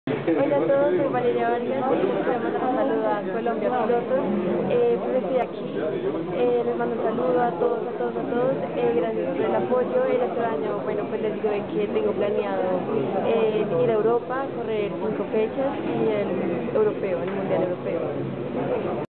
Saludo de la Piloto Colombiana Valeria Vargas a los Fans de ColombiaPilotos
Saludo de  la  Kartista  Colombiana    Valeria  Vargas  a los Fans  de  ColombiaPilotos

► Google + :   Colombia Pilotos